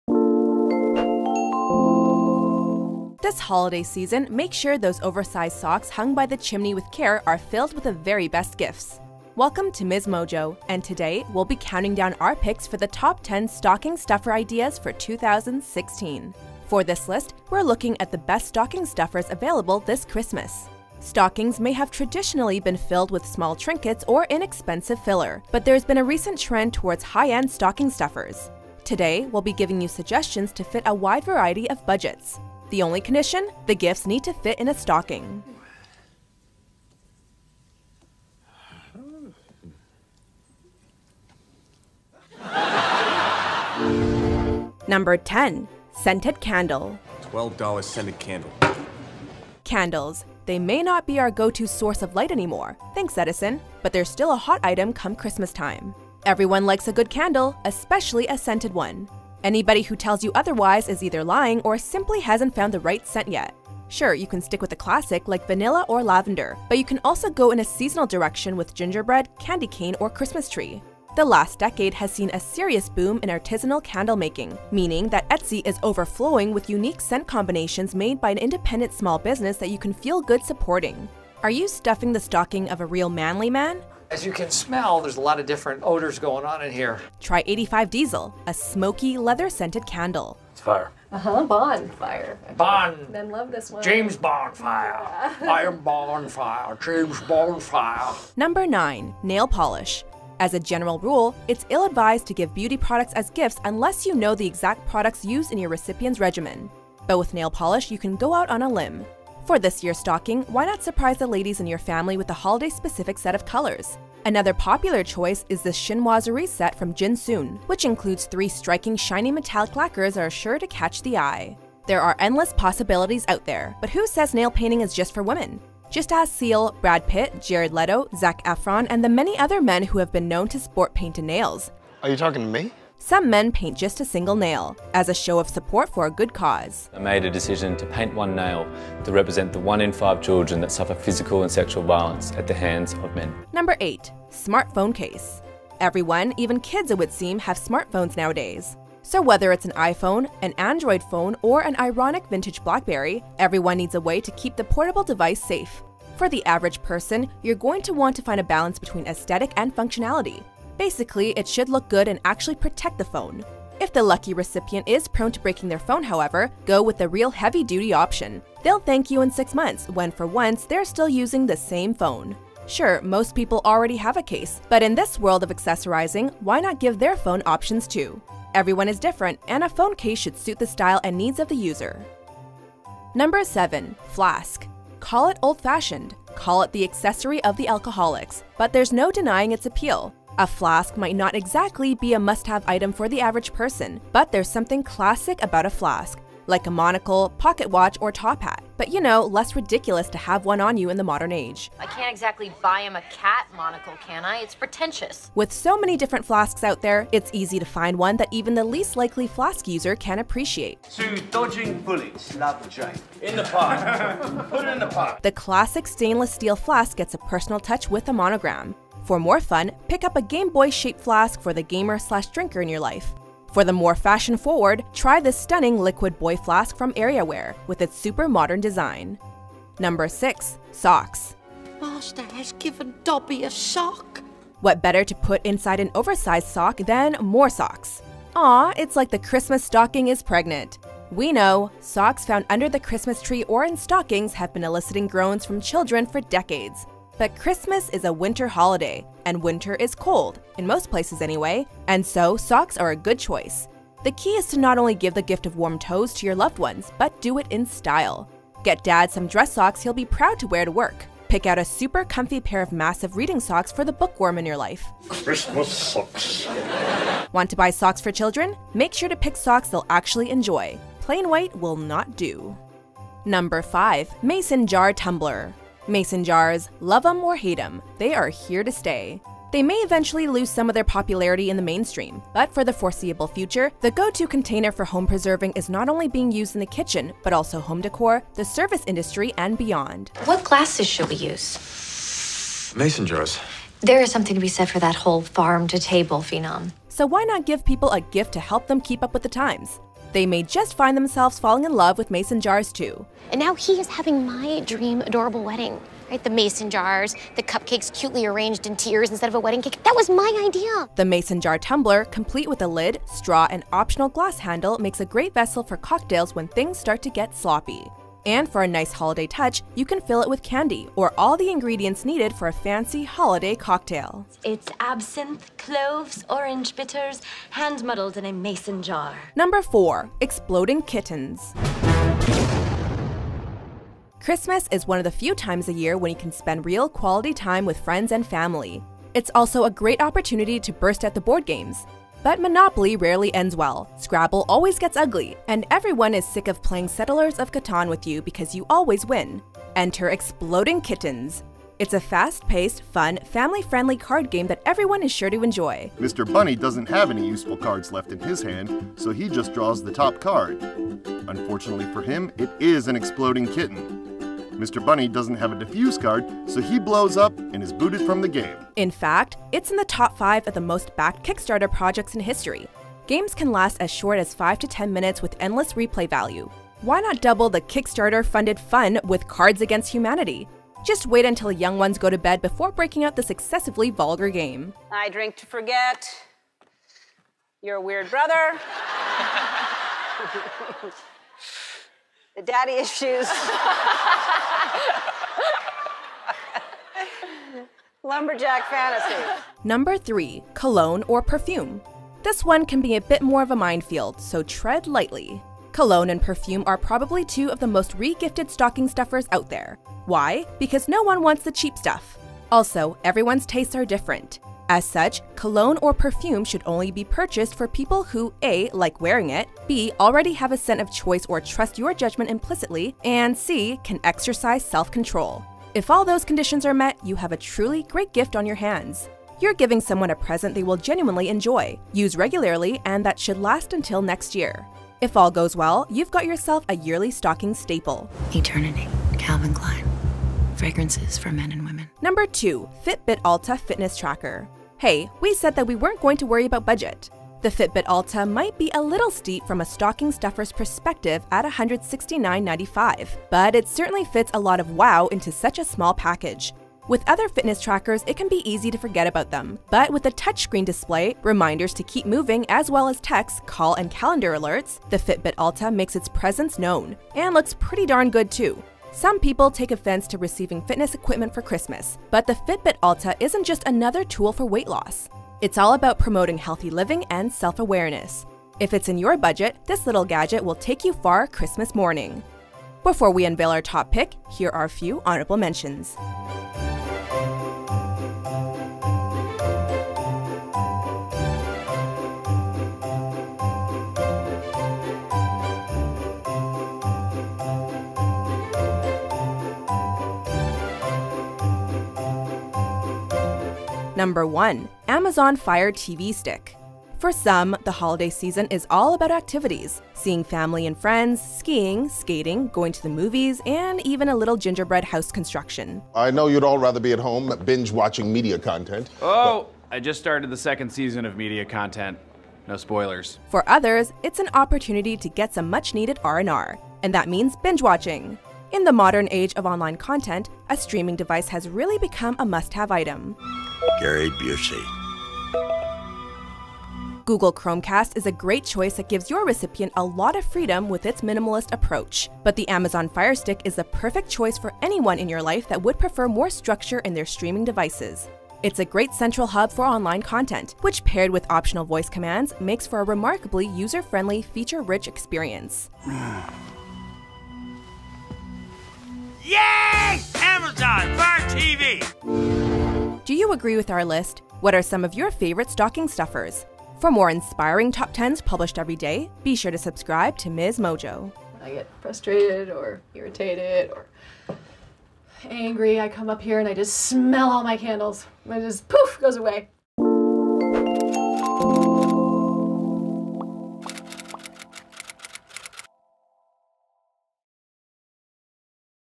Top 10 Stocking Stuffer Ideas
Top 10 Stocking Stuffer Ideas For 2016

Stocking stuffers don’t have to be the same boring things year after year. Sure, we all expect to get socks and chocolate, but there are a myriad of things you can get for your loved ones! In this countdown we’ve come up with a list of Top 10 Christmas stocking stuffer ideas! Some things include scented candles, nail polish, a shiny flask, smart phone case, cologne or perfume, a card game called exploding kittens, a mason jar tumbler, and a Fitbit Alta fitness Tracker.